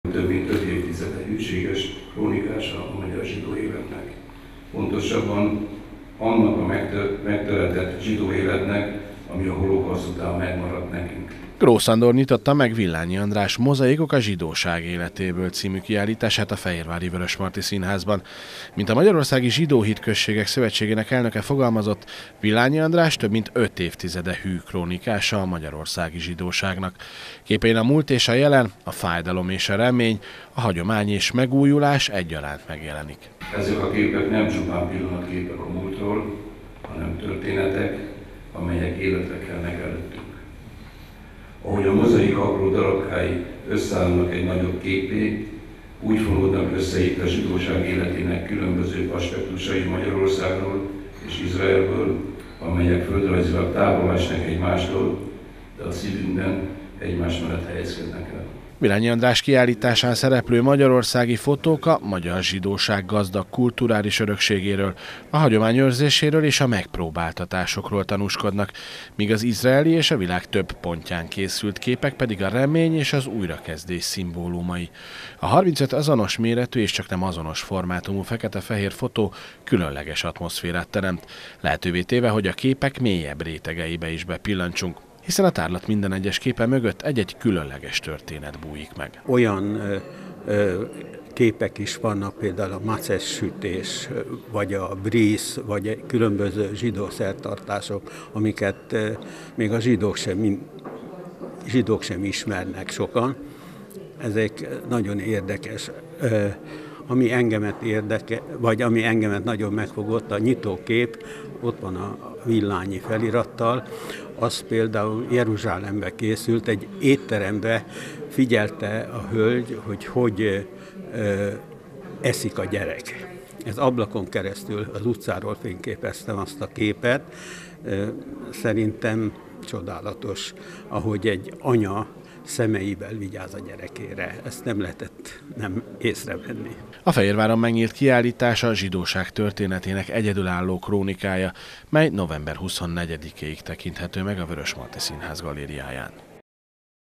MOZAIKOK A ZSIDÓSÁG ÉLETÉBŐL
Gyermekét vigyázó nő, auschwitzban tórát író holokauszt túlélők, vagy épp ünnepre készülő férfiak – csak egy-egy téma, mely szerepel a Mozaikok a zsidóság életéből című fotókiállítás képei közt. Vasárnap megnyílt a magyarországi zsidóság hű krónikásának kiállítása. Villányi András fotóit a Vörösmarty Színház galériáján láthatja november végéig a nagyközönség.